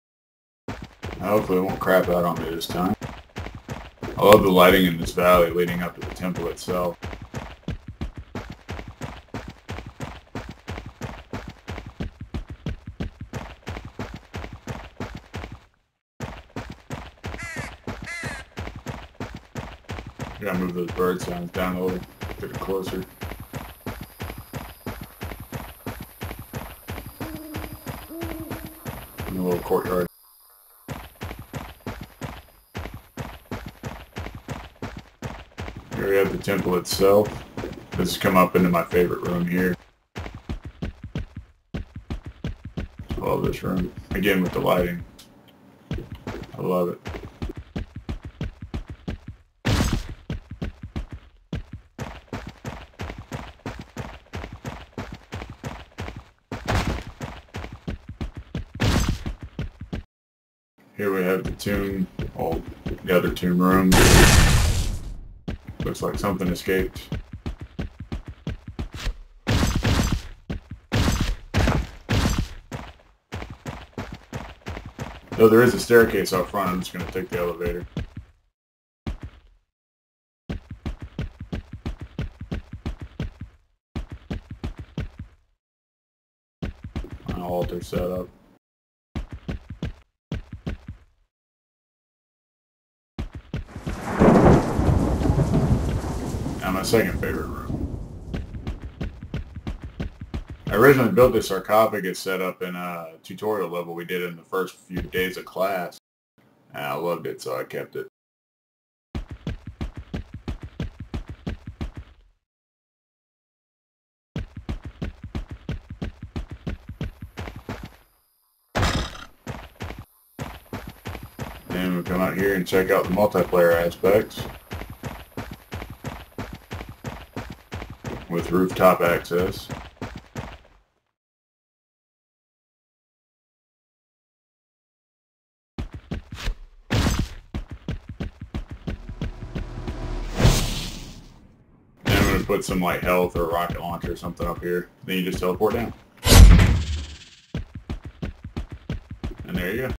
Temple Ruins build 2014
Temple ruins level I made in UDK for my game design course